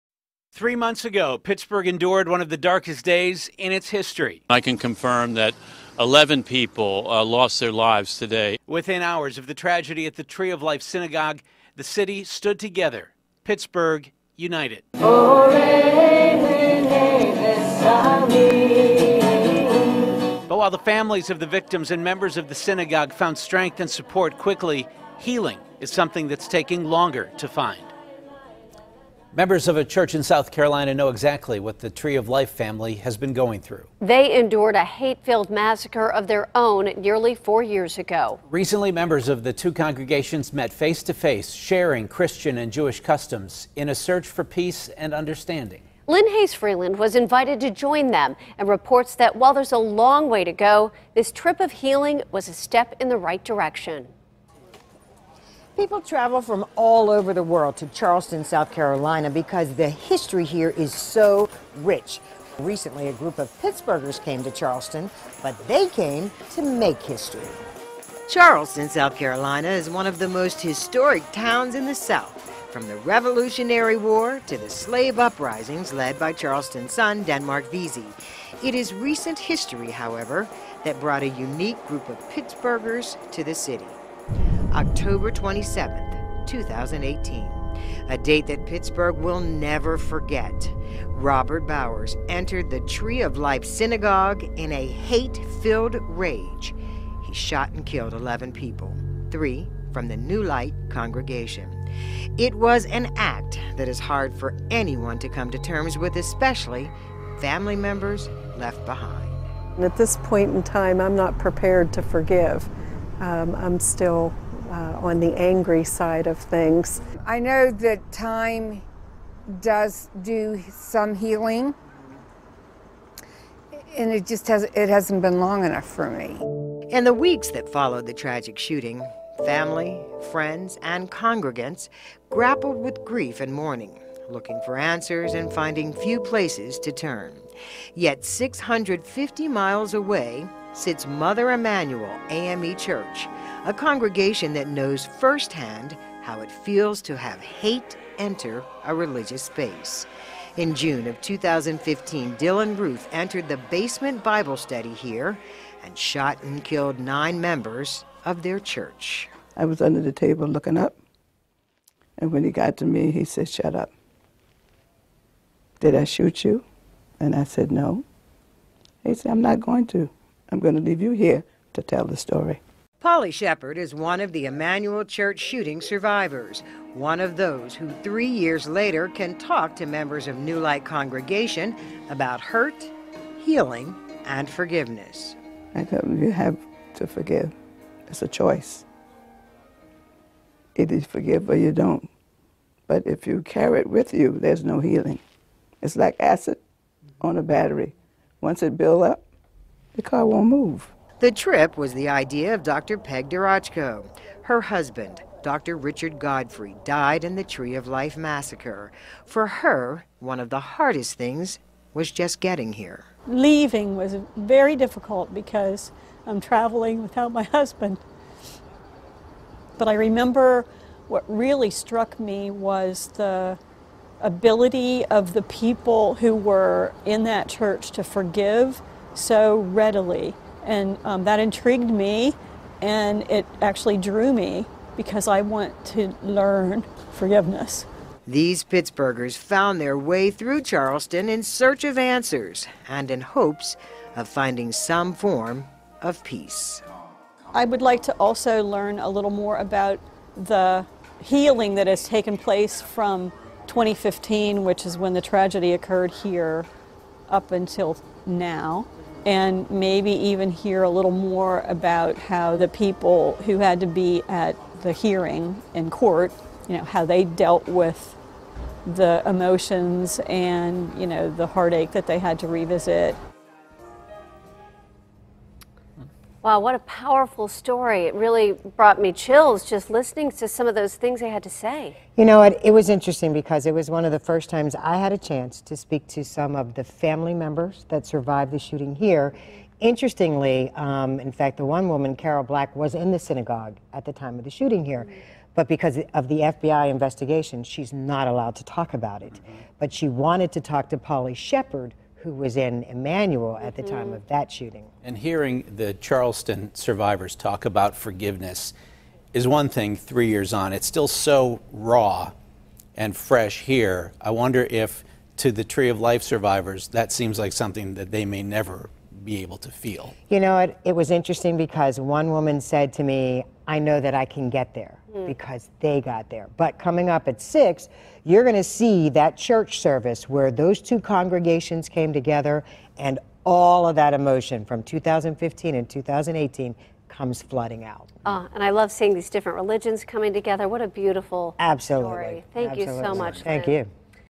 Pittsburghers Travel To South Carolina In Search Of Healing After Synagogue Shooting
Members of the Emanuel AME Church in South Carolina met with members of the Tree of Life family in a search for peace and understanding; KDKA's Lynne Hayes-Freeland reports.